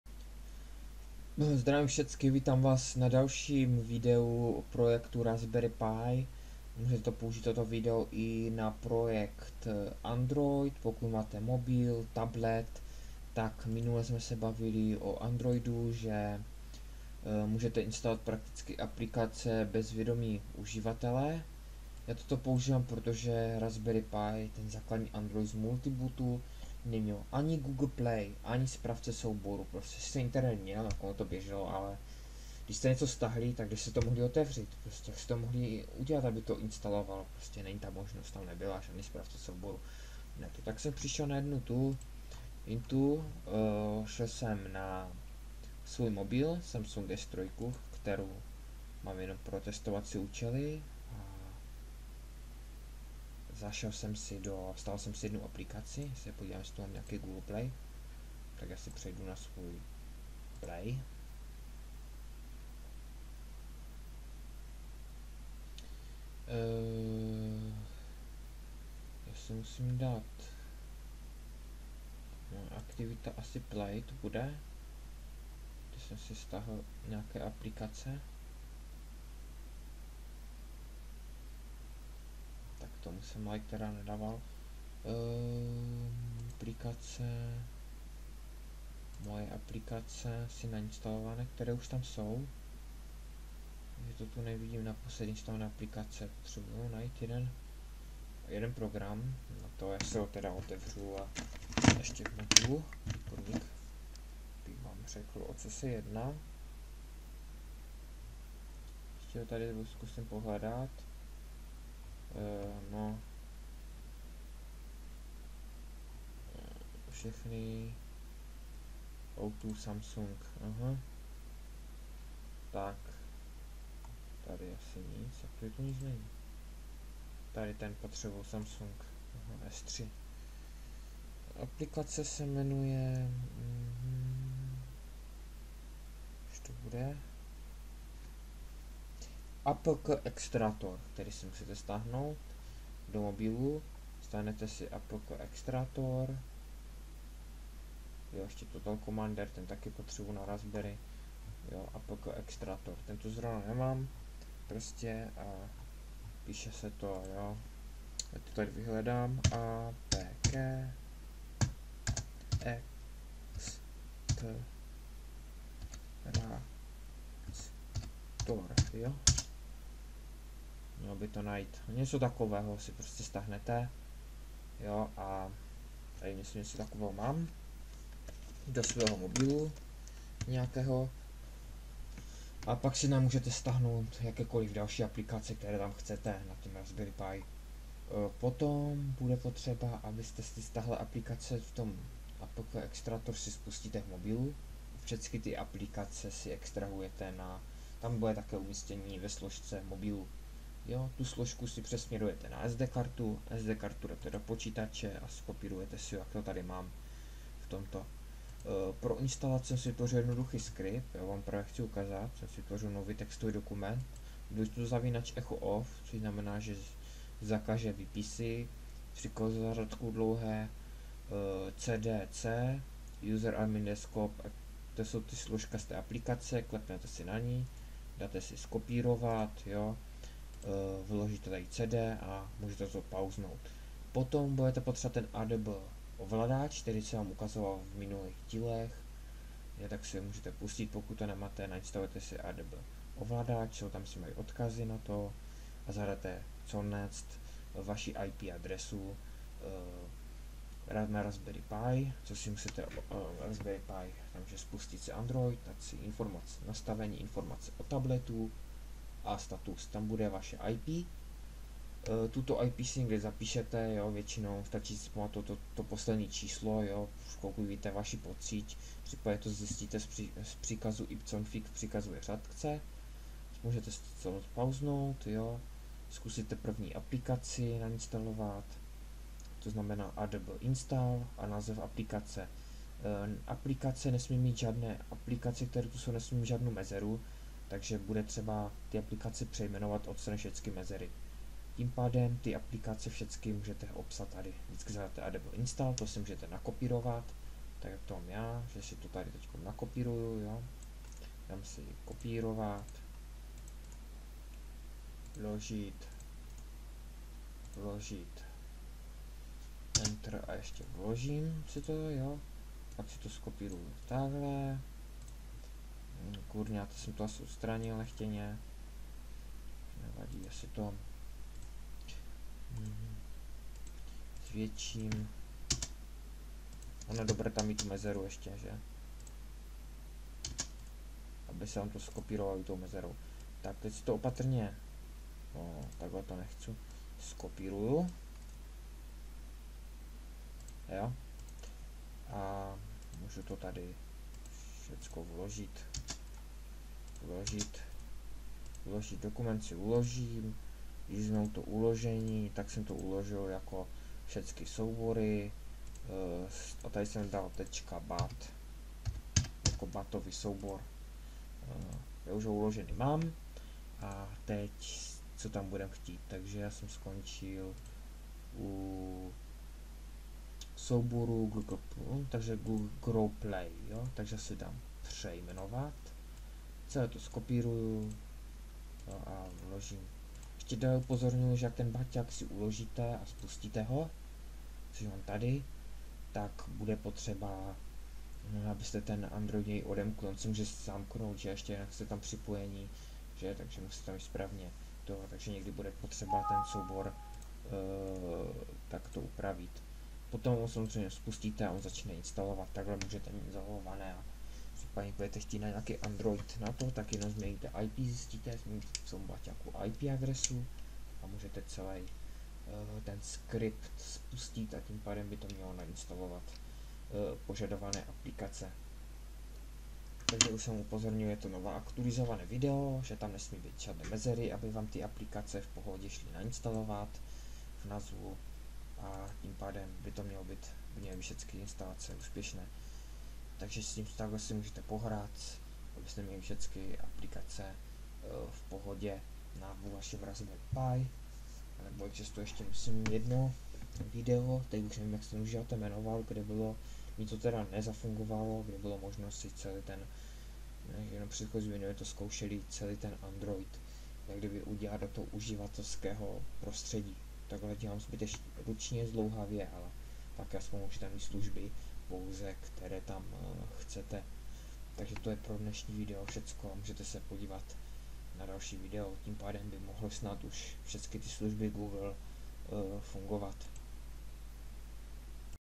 zařizeni s andoidem script pro hromadnou instalaci aplikaci do vašeho zařizení přes wifi sit
zařizeni s andoidem script pro hromadnou instalaci aplikaci do vašeho zařizení přes wifi sit, nebo lan
potřebné je mít adb ovladač
zapnute ladění usb
znát ip adresu zařizení
zakladní znalosti s windows, linux prace se soubory a složky
zařizení s androidem mobil, tablet raspberry pi 1, raspberry pi 2

android install akp witch wifi hack